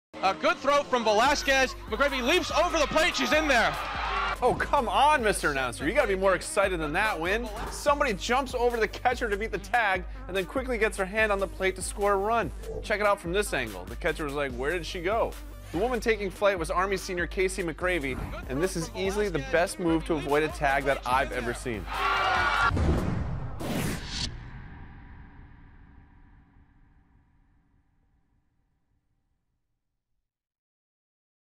Army softball player jumps over catcher to score run
Well, this is one very awesome way to avoid the tag at home plate.